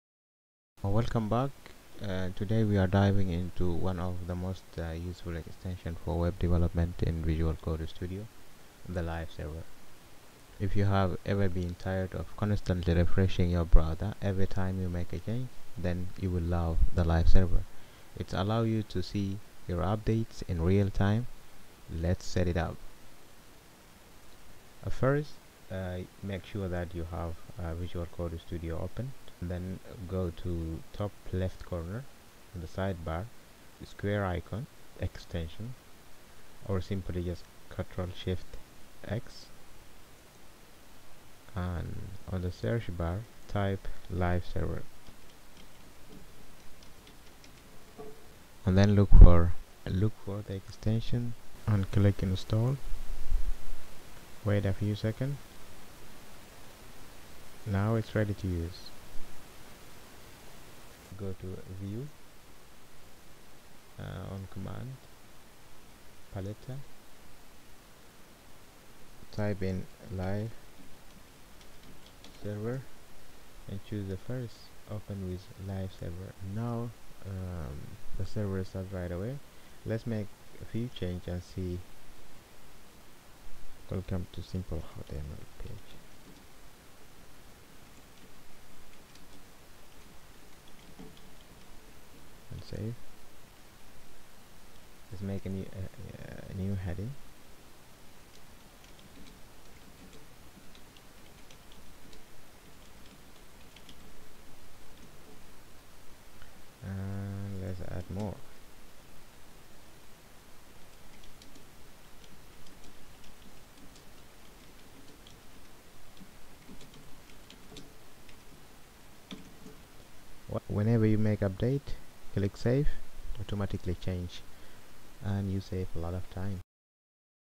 Visual Studio Code Tutorial: Setting Up the Live Server
🚀 Welcome to this tutorial on setting up the Live Server extension in Visual Studio Code! 🚀
In this step-by-step guide, we'll dive deep into one of the most useful tools for web developers: the Live Server. Say goodbye to the constant back-and-forth between your code and browser. With Live Server, every change you make gets instantly reflected, offering a real-time preview of your website.

 🔍 What you'll learn:
- How to install the Live Server extension in VS Code.
- Initiating a real-time preview of your website.
💡 Why Live Server?
Live Server automates browser reloading, ensuring a smooth and time-saving coding experience.